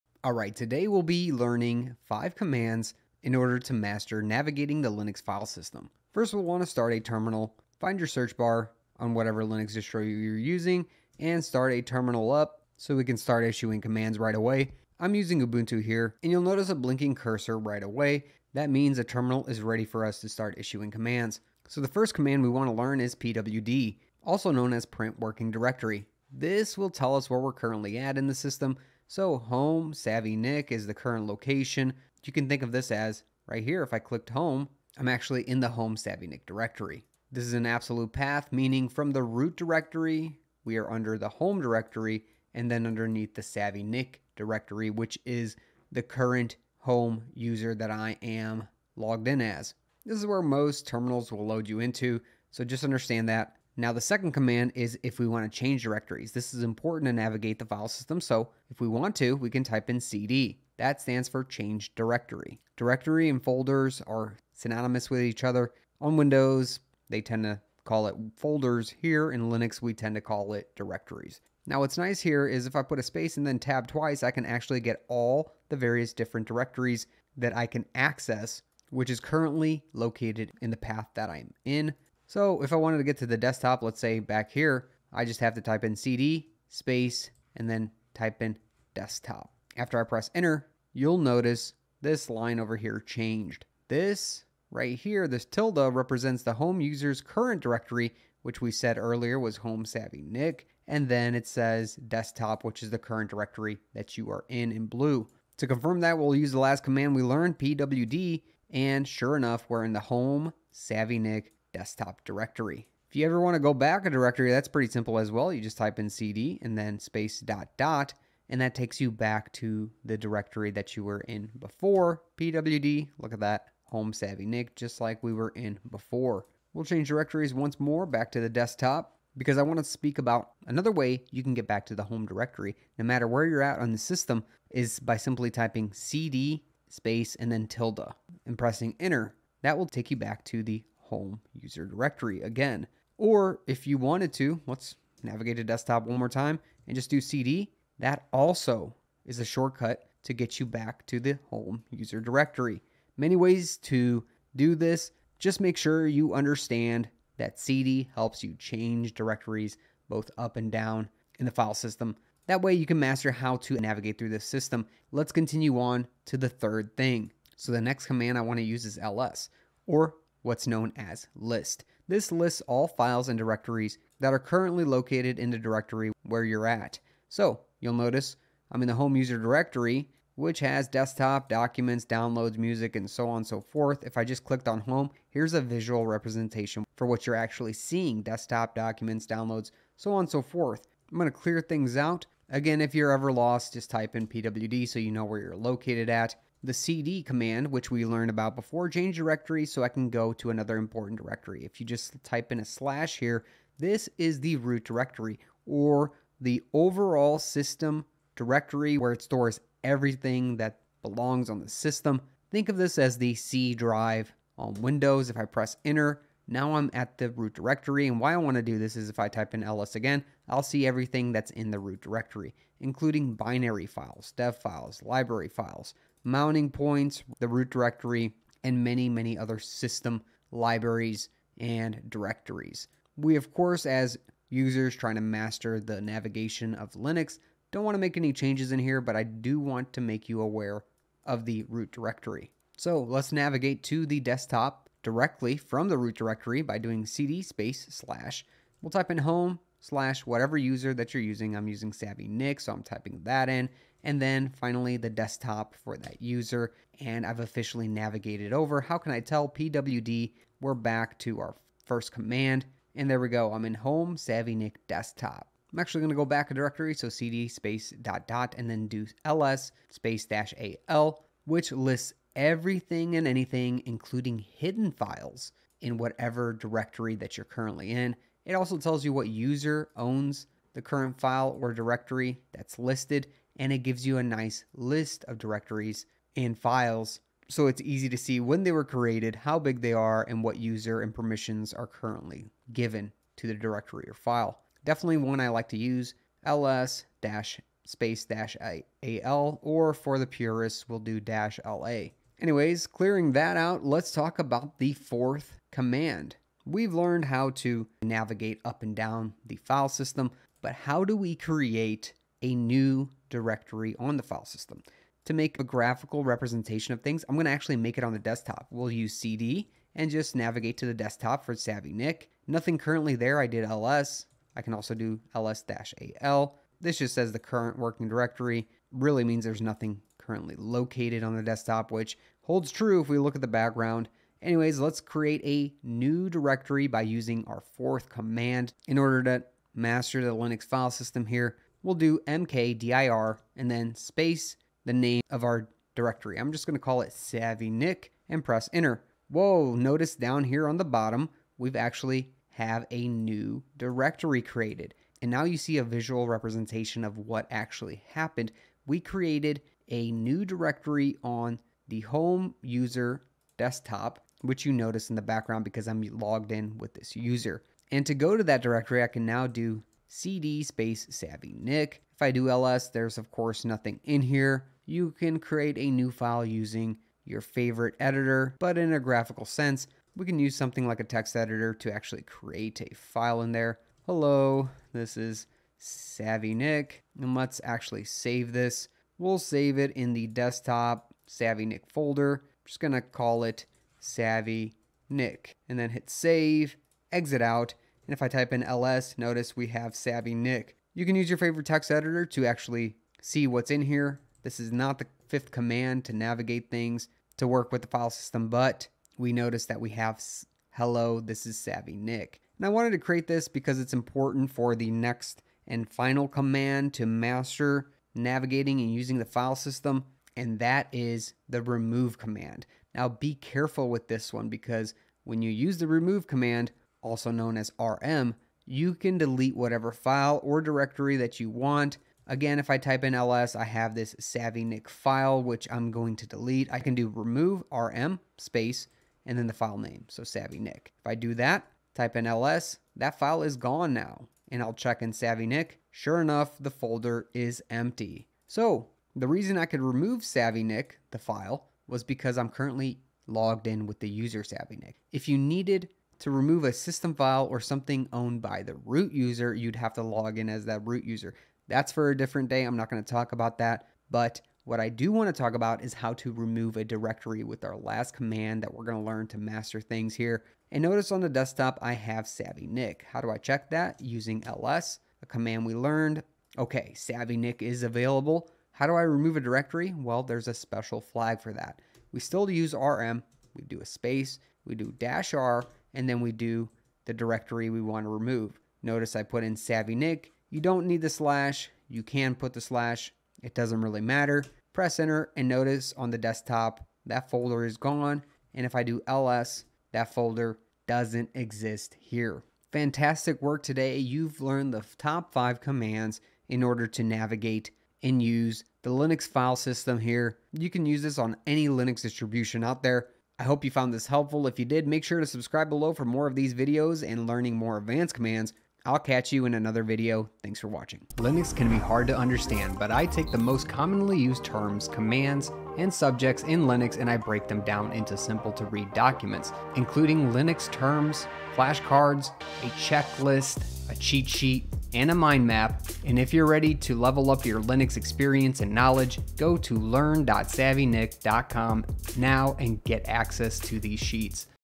How to use the Linux File System with 5 EASY Commands.
Use these terminal commands to master navigating the Linux File System. These 5 commands are essential and the absolute basics for beginners to start using the linux command line and on how to use Linux.